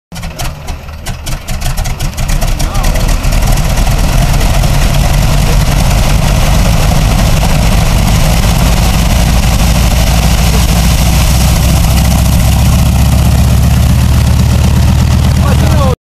Canadian Harvard Aircraft Startup
Beautiful bird, nice sounding startup with this radial engine!